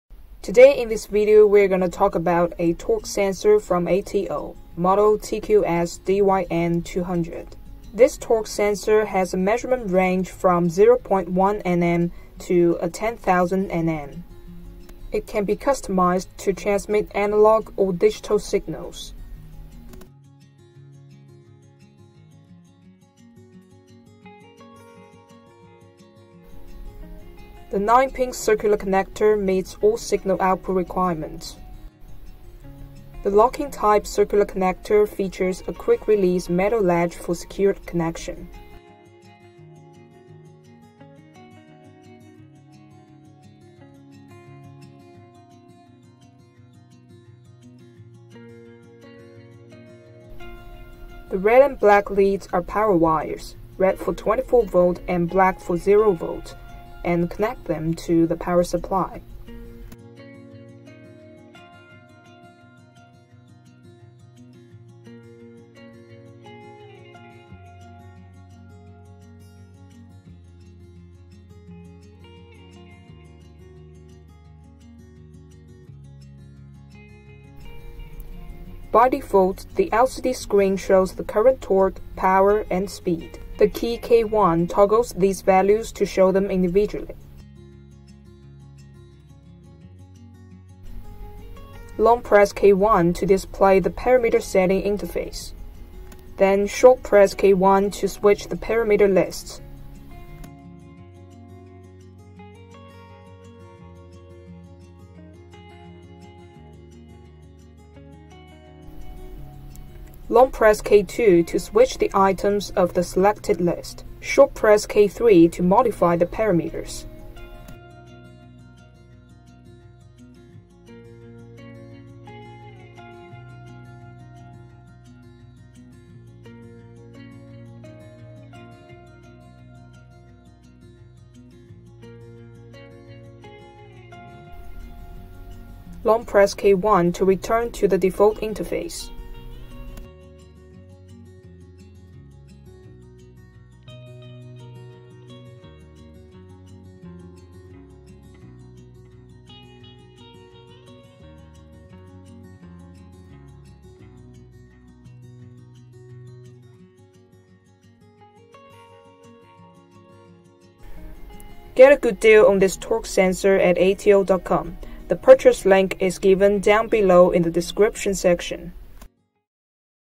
Digital Torque Sensor Wiring & Setting
This torque sensor feature a wide capacity range from 0.1/5/300/1000 Nm to 10000 Nm, with optional RS485 as well as other types output signals. Equipped with a 7 pin waterproof circular locking connector and 3m cable, the torque sensor can measure both forward and reverse torque.